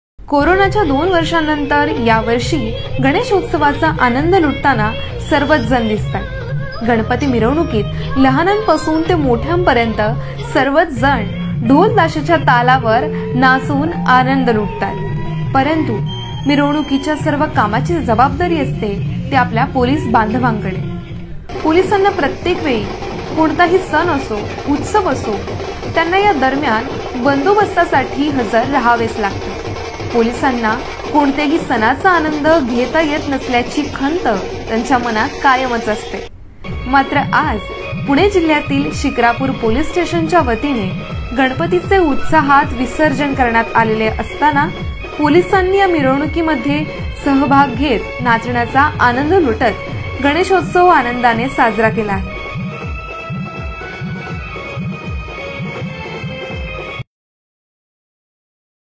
Ganeshotsav 2022 : पोलीस बांधवानी घेतला मिरवणुकीत नाचण्याचा मनमुराद आनंद
शिक्रापूर (प्रतिनिधी) - पोलिसांना कोणतेच सण, उत्सव साजरे करण्याचा आनंद घेता येत नसतो. प्रत्येकवेळी कोणत्याही उत्सवावेळी पोलिसांची सुट्टी देखील रद्द होत त्यांना बंदोबस्तावर हजर रहावे लागते. मात्र, शिक्रापूर पोलीस स्टेशनच्या पोलिसांनी पोलीस स्टेशनमध्ये बसवलेल्या गणपतीचे मोठ्या उत्साहात विसर्जन करीत आनंद लुटला.